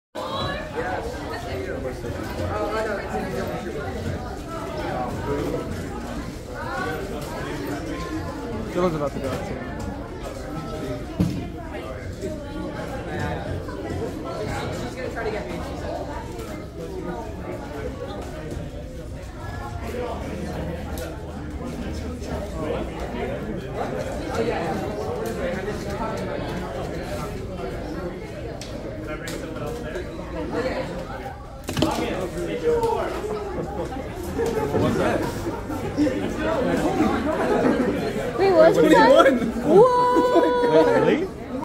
21.81 seconds H2OH XWR at UCSD Unofficial Events 2023
H2OH is an unofficial event where you try to drink a bottled water and solve a cube OH as fast as you can!  I am technically the world record holder with this result!

Thank you to Elana for filming and Dylan for video editing!